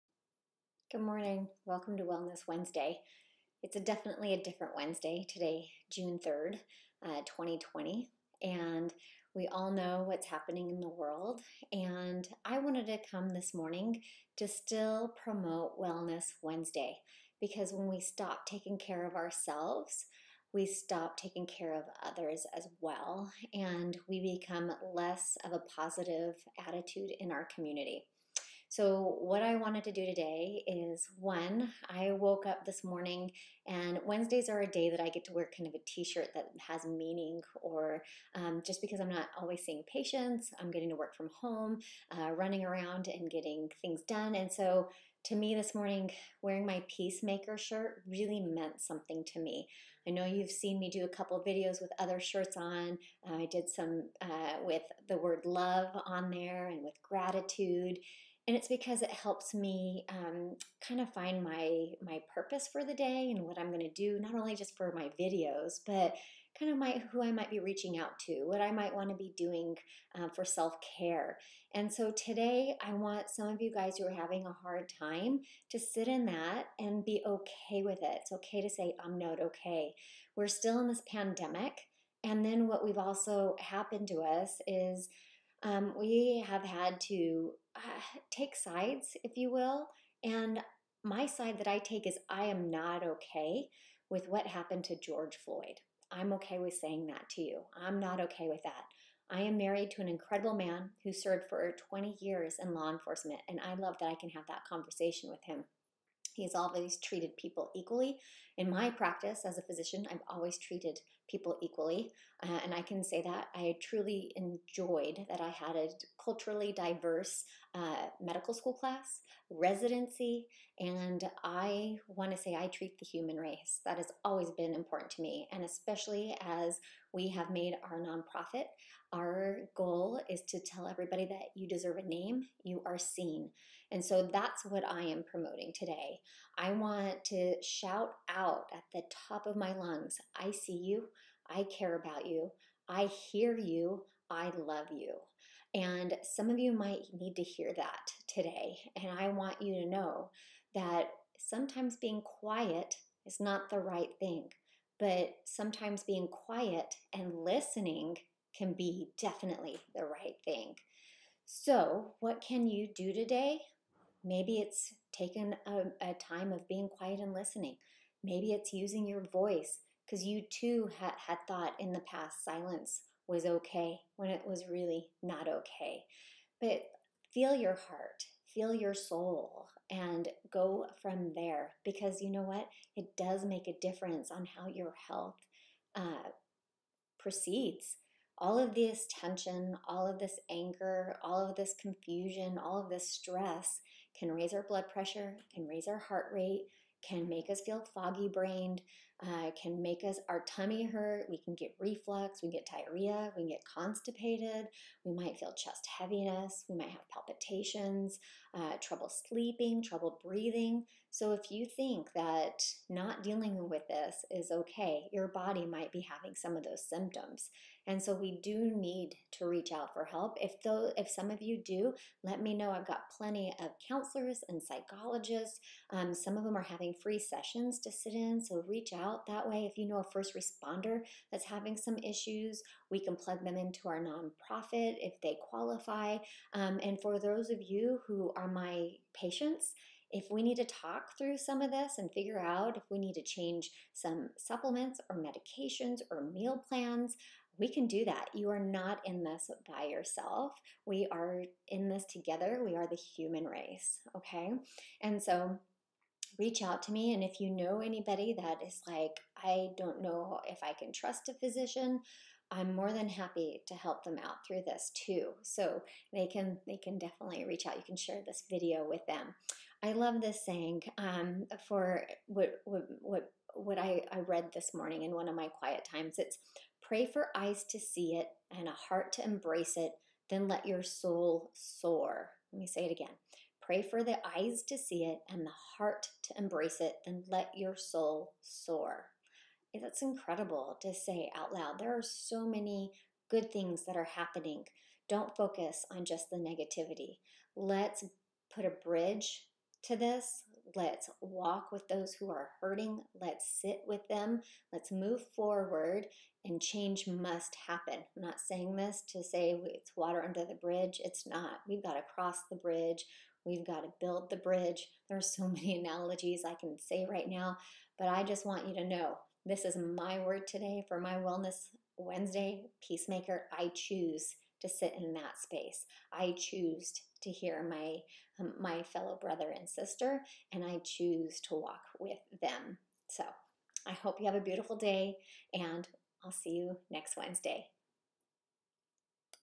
Wellness Wednesday - Peacemaker
Lean in and listen as Dr. Rodarte touches on current events and how it impacts wellness.